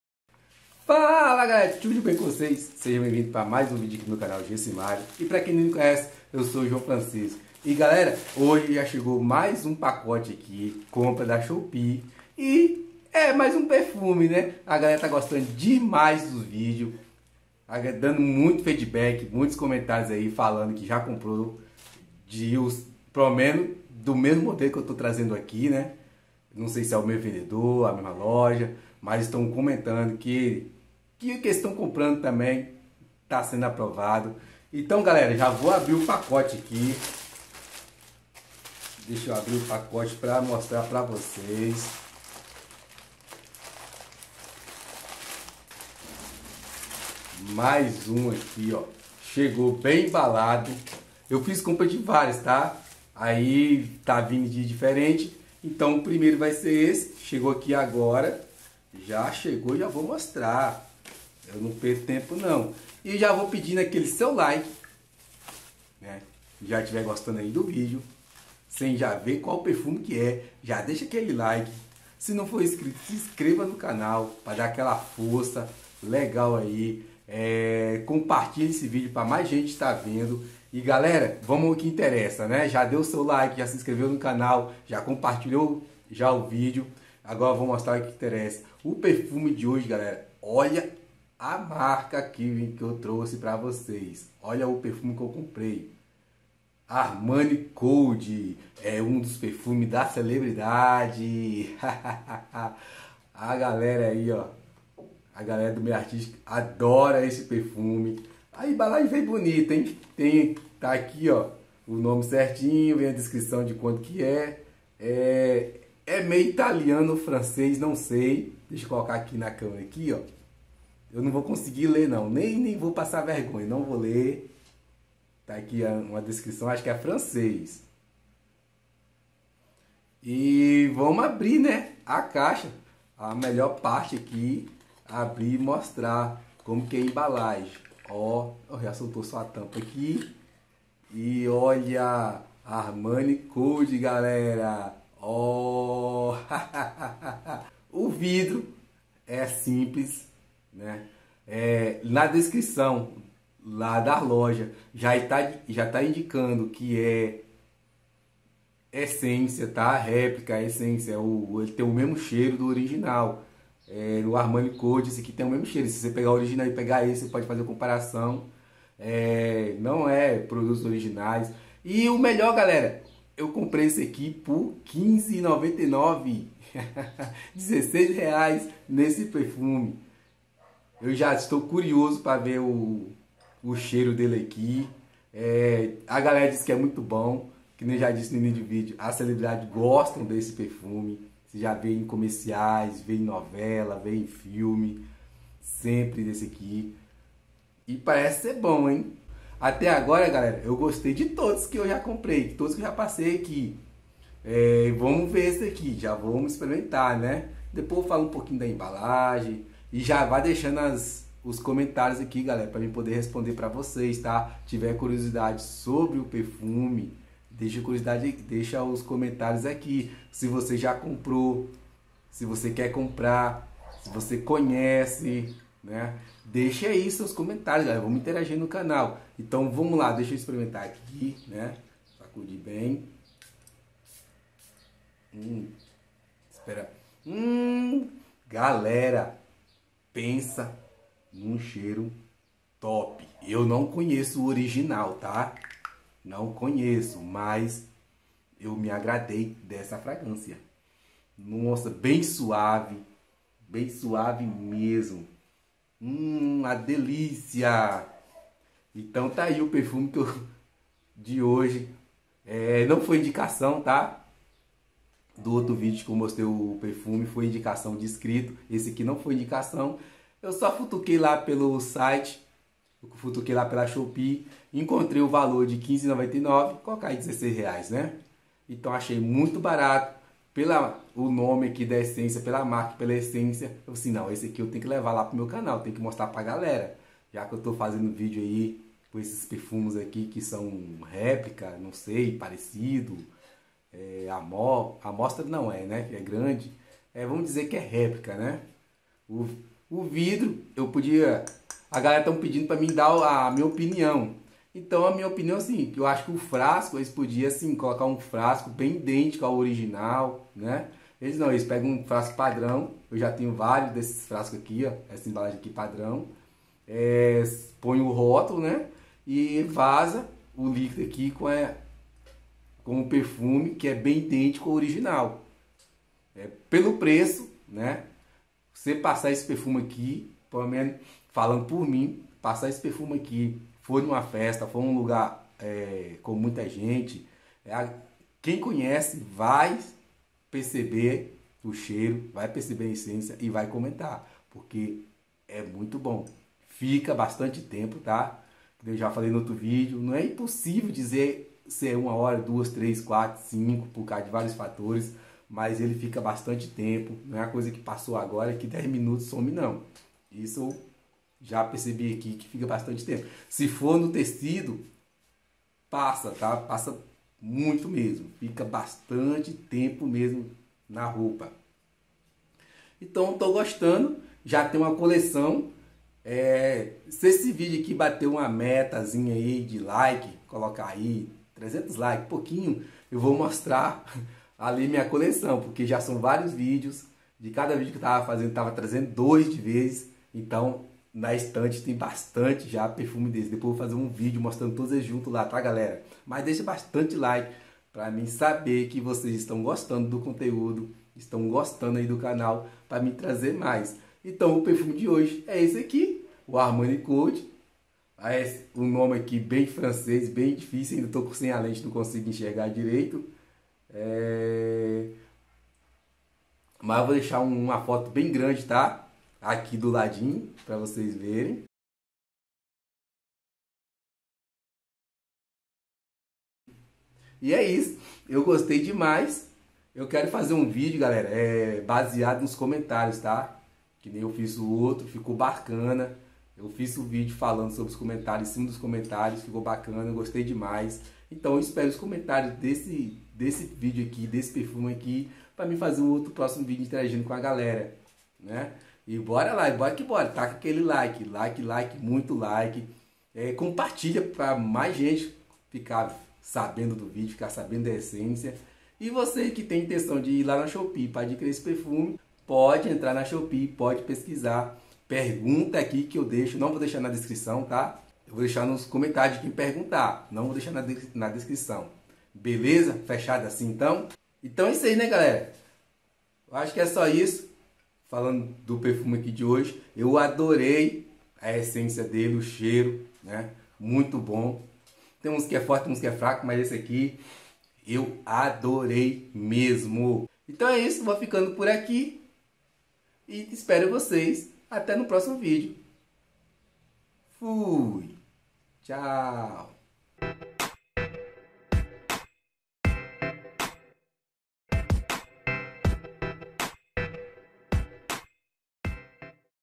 Perfume Armani Code na Shopee por 15,99
Salve galega no vídeo de hoje vou apresentar mas um perfume de essência  importado comprado por 15,99  veja o vídeo e deixa se like
Seja membro deste canal e ganhe benefícios: